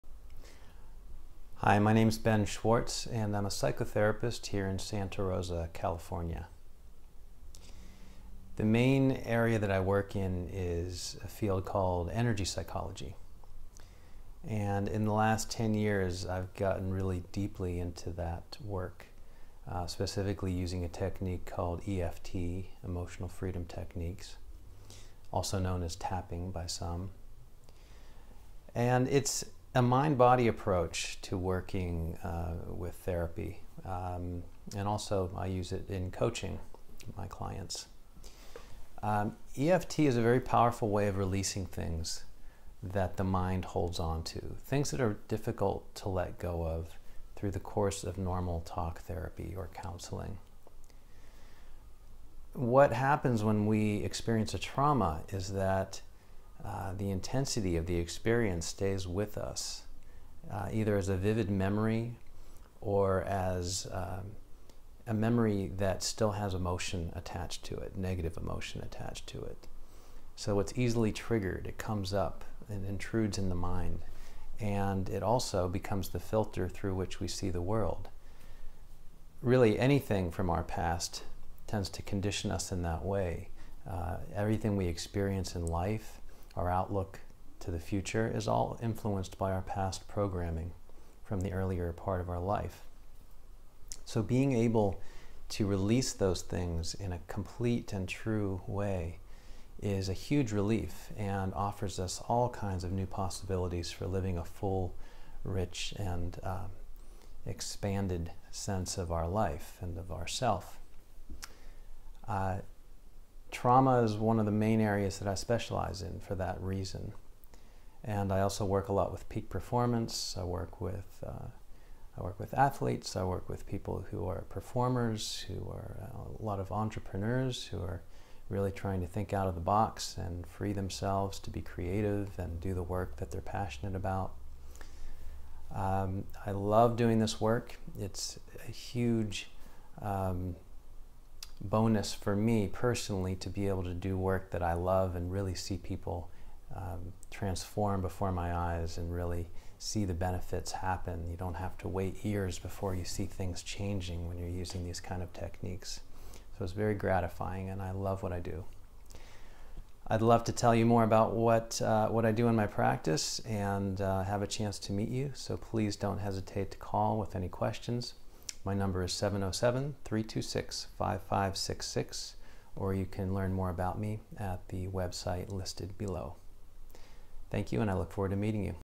Santa Rosa Psychotherapist and Transformational Coach
Hi I'm Ben Schwarcz, Santa Rosa Psychotherapist, Life Coach and Certified Advanced EFT Tapping Practitioner and Trainer.

To get a free no-obligation assessment of your current life situation and a plan of how you can alleviate emotional pain, let go of past traumas once and for all, and experience better relationships, success and flow in your life, call me at .

I can help you to:
Release the mental and emotional blocks that are keeping you    stressed, frustrated and stuck...
Find the easier path to get what you want in life without self-sabotage, resistance and fear...
Help you to release and transcend your self-limiting beliefs...
Attract more positive people and experiences into your life.

EFT Tapping is a powerful energy based, mind-body approach for releasing stress, anxiety, and trauma. Tapping can deepen the therapy process, allowing for a more complete release of negative emotions, and can help to program the subconscious to allow more happiness, connection, and flow. For peak performance in all areas of life.

I have conducted thousands of sessions using EFT Tapping, and meditation, and also offer hypnotherapy and other mind-body techniques for accelerating your personal growth. I would love to help you create more joy, more peace and more freedom, for yourself, and for the world.

Santa Rosa Psychotherapist